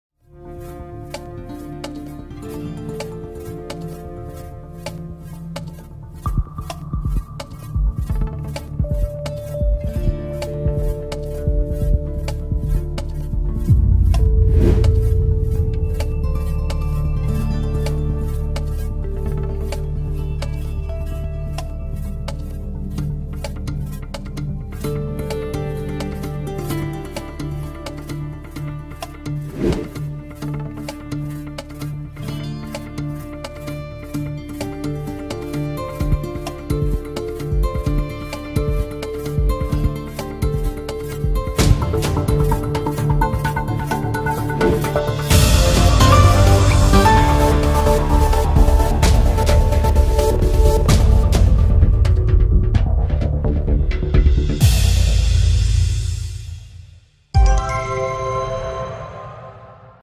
Глава республики в прямом эфире. Героическая история врача. Новости Мордовии
- Героическая история врача из Мордовии;
- Артём Здунов выйдет в прямой эфир;
- Мордовское искусство покоряет Москву;
- Жарил картошку - спалил всю квартиру